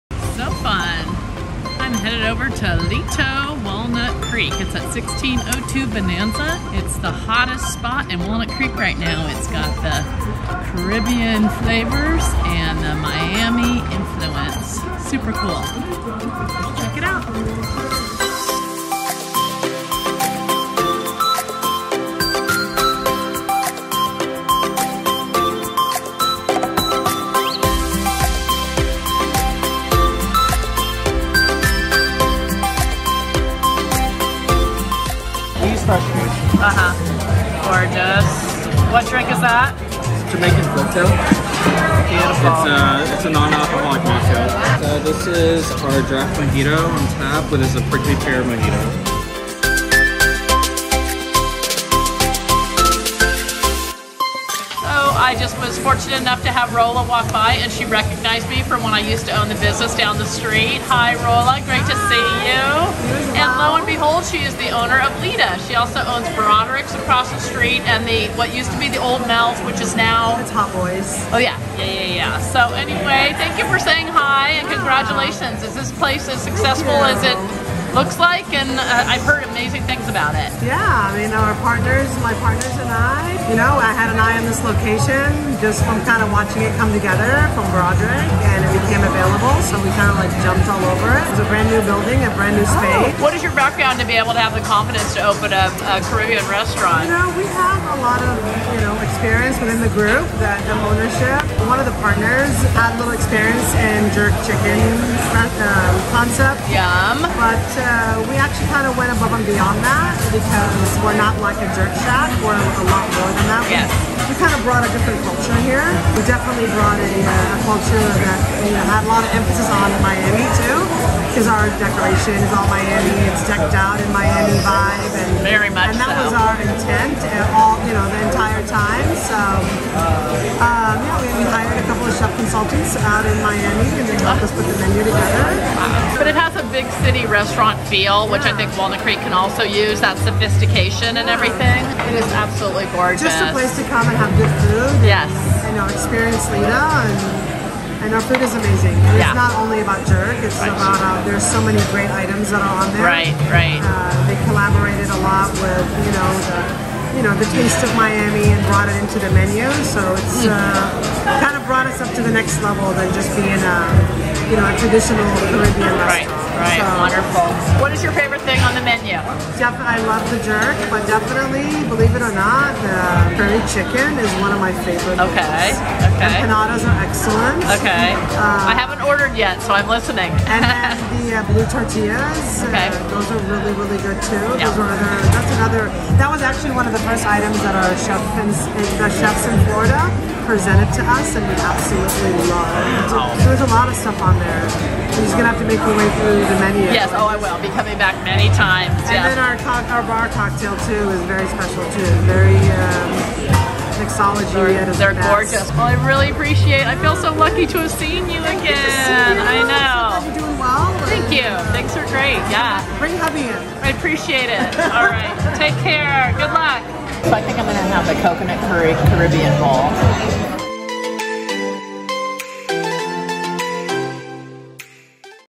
Check out Lita in Downtown Walnut Creek!
LITA, is a modern Latin-Caribbean restaurant located in the heart of Walnut Creek's downtown. With interiors inspired by Miami's diverse art scene and vibrant cultural influences, LITA is a MUST visit!

𝒮𝓉𝑒𝓅𝒽𝒶𝓃𝒾𝑒 𝑀𝒶𝓇𝓇𝑜𝓃𝑒
DRE# 02115267